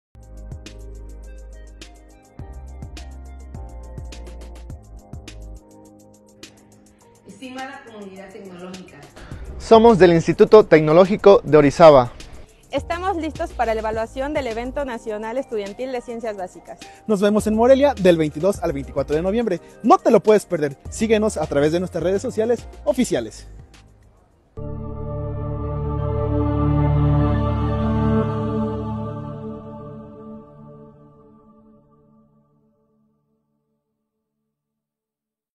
ENECB 2023 campus Orizaba
¡El Evento Nacional Estudiantil de Ciencias Básicas 2023 está por arrancar!

La delegación del TECNM Campus Orizaba, se encuentra lista para la Evaluación Nacional que no te puedes perder 22 al 24 de noviembre.

Facebook 👉 goo.su/rMvRceU
Twitter 👉 goo.su/d0ga
YouTube 👉 goo.su/kPa0vQ